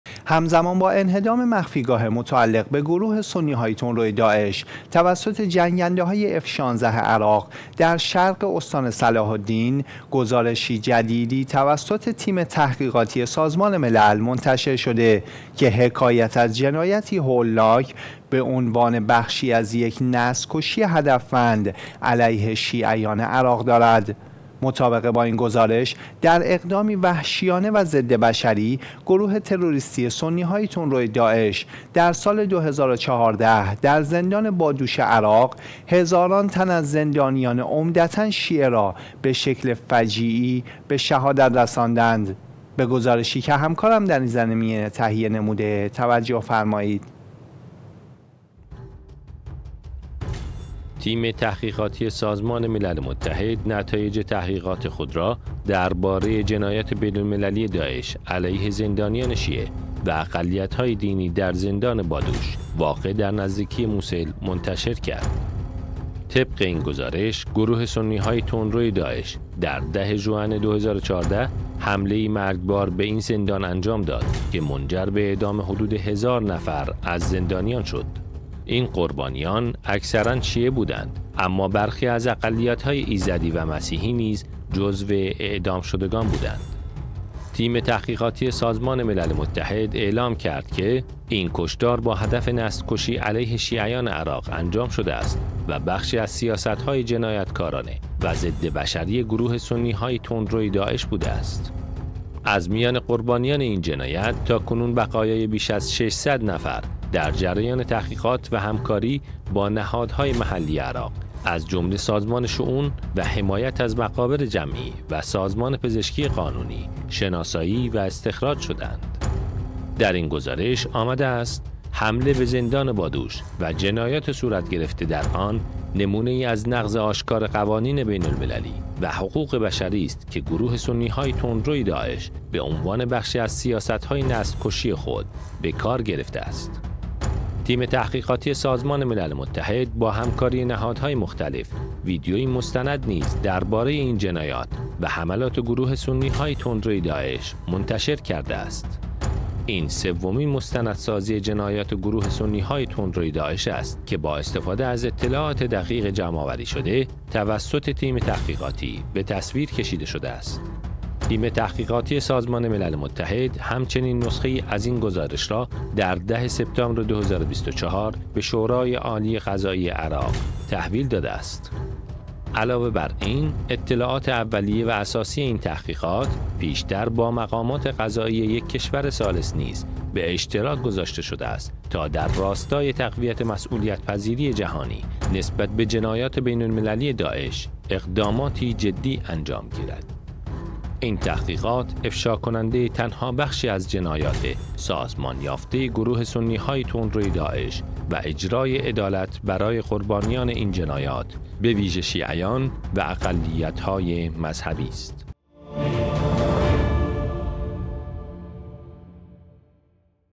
نسل‌ کشی مخفی: داستان جنایات داعش علیه زندانیان شیعه
در این ویدیو، پرده از جنایات هولناک و نسل‌ کشی مخفی گروه تروریستی داعش علیه زندانیان شیعه در زندان بادوش عراق برداشته می‌شود. سازمان ملل متحد در گزارشی تکان‌دهنده، به افشای جنایات جنگی داعش و اعدام‌های دسته‌جمعی پرداخته که به عنوان بخشی از سیاست‌های جنایتکارانه و نقض آشکار حقوق بشر توسط این گروه سنی‌های تندرو علیه شیعیان و اقلیت‌های دینی در عراق صورت گرفته است. این مستندسازی شامل اطلاعات دقیق و تحقیقات تیم‌های بین‌المللی است که در شورای عالی قضایی عراق نیز مطرح شده‌اند.

📌 تماشا کنید و به اشتراک بگذارید تا صدای عدالت برای قربانیان این جنایات بین‌المللی، به ویژه شیعیان و دیگر اقلیت‌های دینی، به گوش جهانیان برسد.
برای عضویت، به لینک زیر مراجعه فرمایید👇
00:00 - مقدمه: معرفی جنایات داعش علیه زندانیان شیعه
02:30 - نقش سازمان ملل در تحقیق درباره جنایات جنگی
05:45 - مستندسازی جنایات داعش در زندان بادوش
08:20 - اهمیت عدالت بین‌المللی برای قربانیان